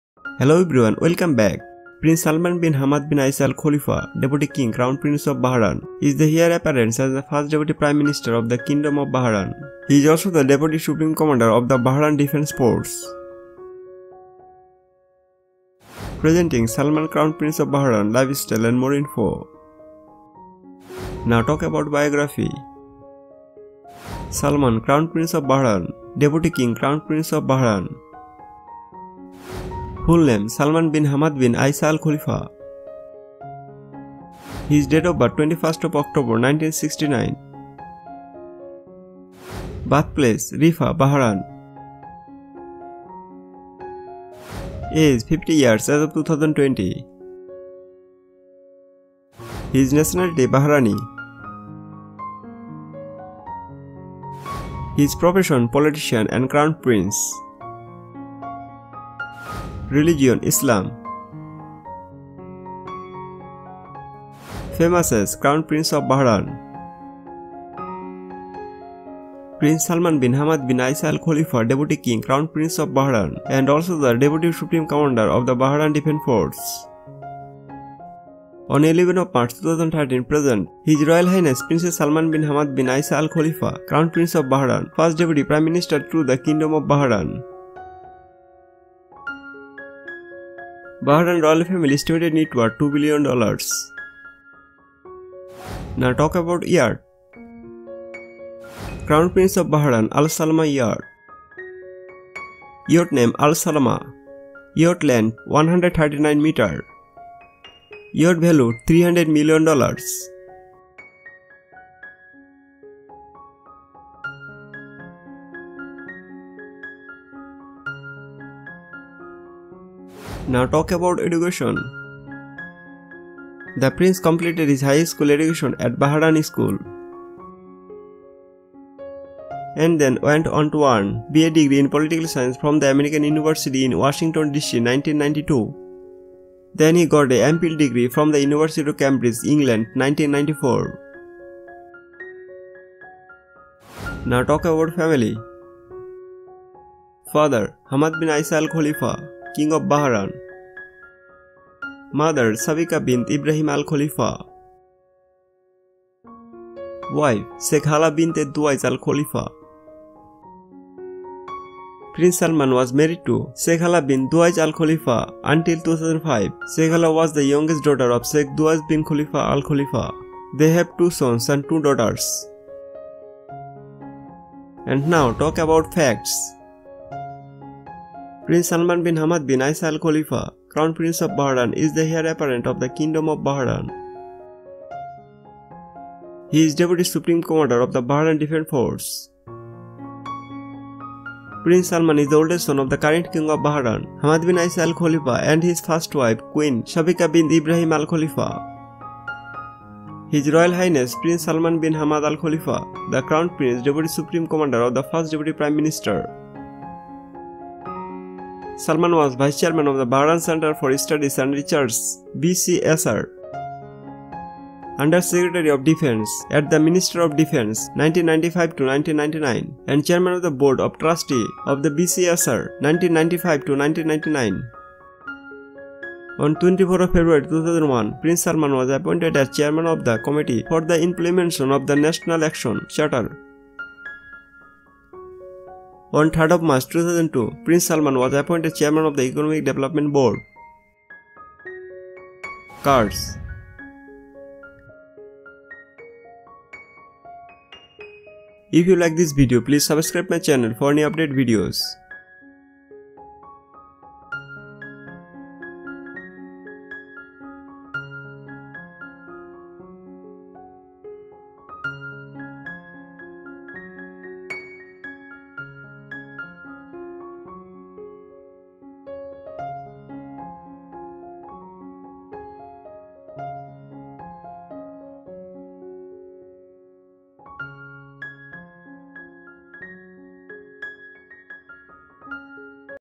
Salman, Crown Prince of Bahrain Lifestyle★Bio★Family★Yacht★Education★Facts★Net Worth & More Info
Welcome back,Prince Salman bin Hamad bin Isa Al Khalifa, Deputy King, Crown Prince of Bahrain is the heir apparent and first deputy prime minister of the Kingdom of Bahrain. He is also the deputy supreme commander of the Bahrain Defense Force.